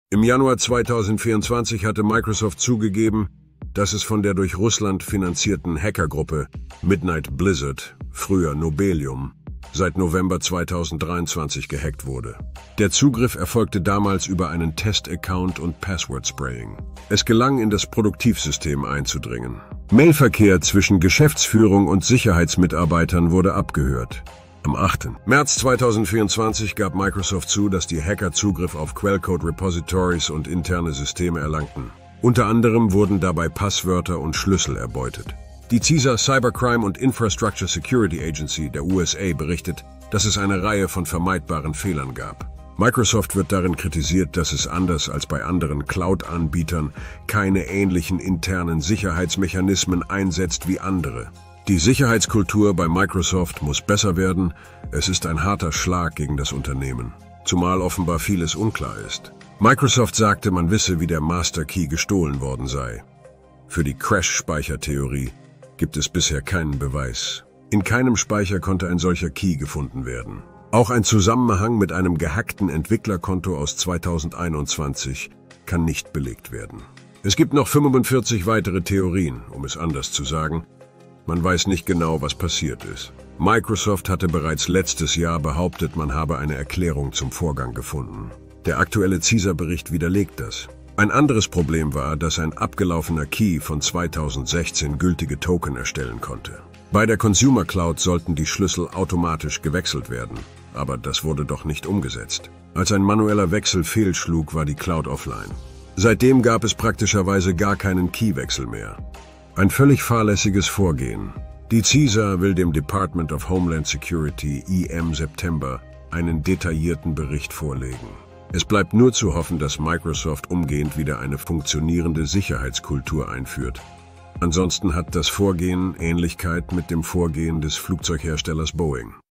Microsoft von Russland gehackt - Katastrophe
Bereits seit November 2023 wird Microsoft von russischen Hackern angegriffen. Die Angriffe haben noch kein Ende, nicht zuletzt durch überraschend dürftige Sicherheitsarchitektur.